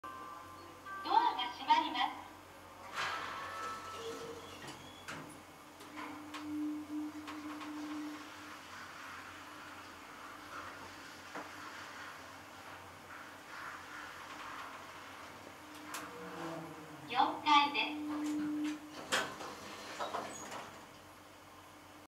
北海道エレベーター動画（小樽市）4K対応その26 Videos taken in Hokkaido
小樽市にある朝里川温泉ホテルのエレベーター（東芝エレベータ製）の動画です。２０２３年７月１日撮影。
Elevator in Otaru City part 26